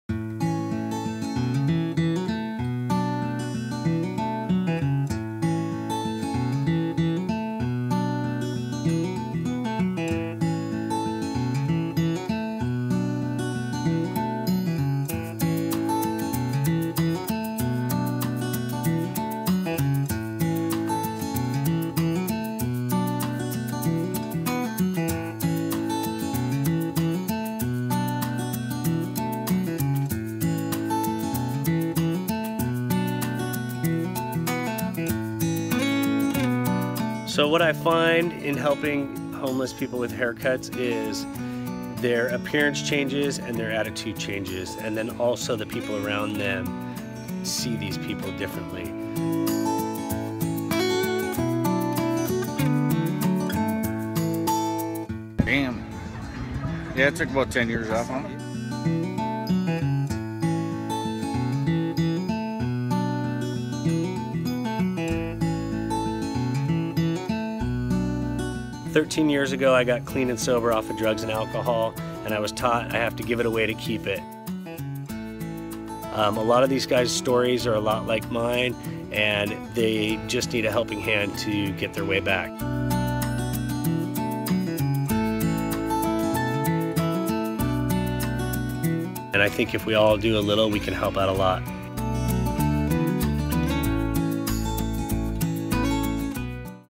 Barber Gives Homeless Free Cuts
Jason Schneidman usually styles the hair of A-List clients. This barber takes his talent to LA's streets, empowering the city's homeless with a free hair cut. Schneidman hopes to bring awareness to the city's homeless population of 60,000 people.